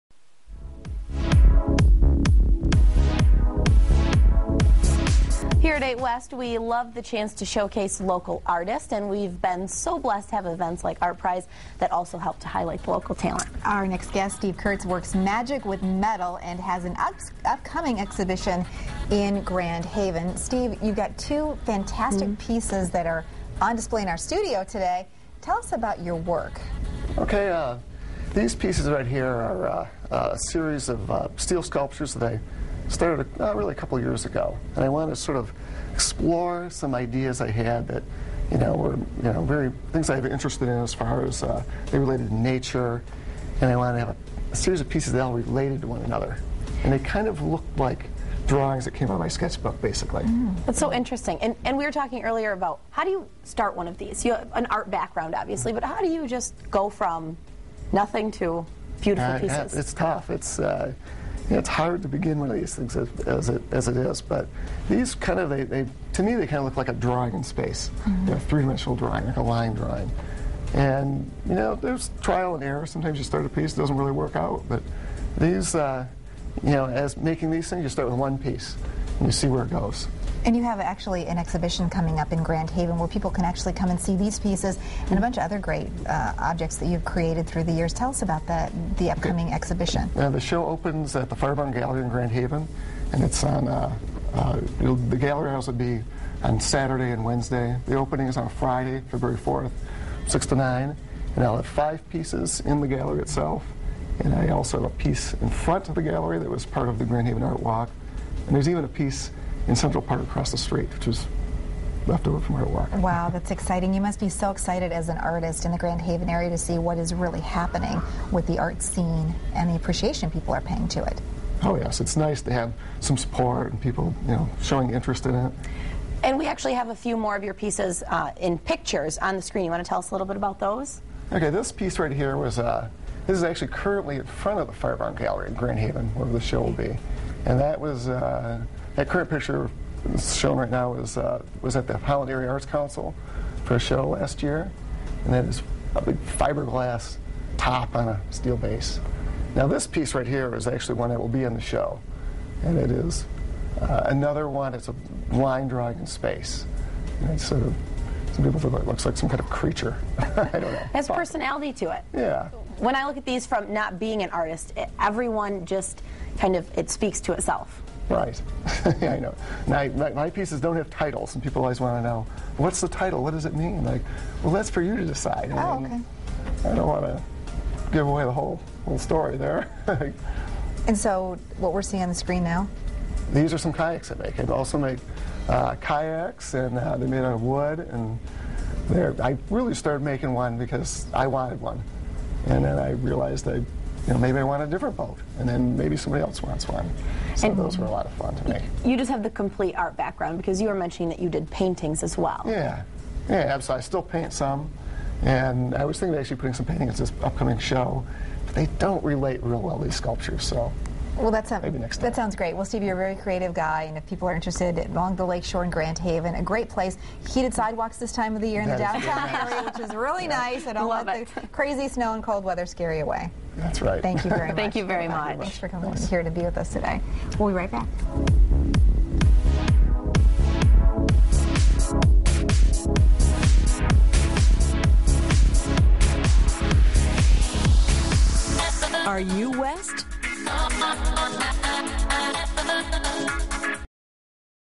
Working magic with metal
Works of art by Grand Haven artist Steve Kurtz will soon be on display at the Fire Barn Art Gallery